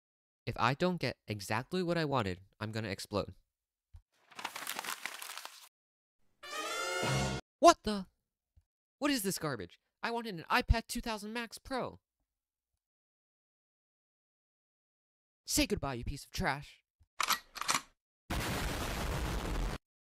Rich Kids on Christmas
I recorded my voice using a new microphone I got for Christmas (Audio-Technica AT2005USB)